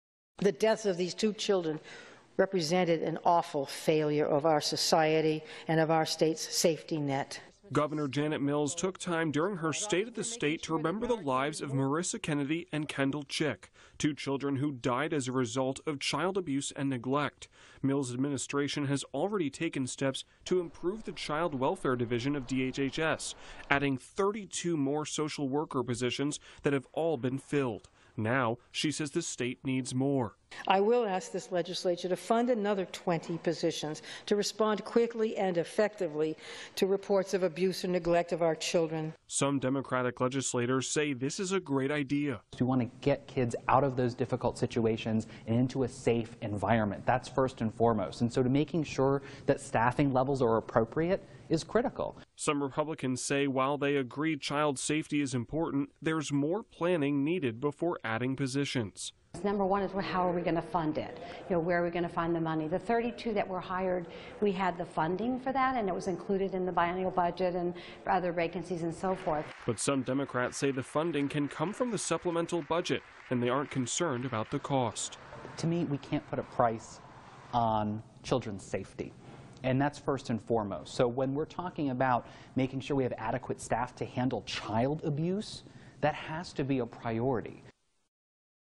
Governor Janet Mills: Adding staff to protect children
Commenting on the Governor's proposal as chairman of the Government Oversight Committee.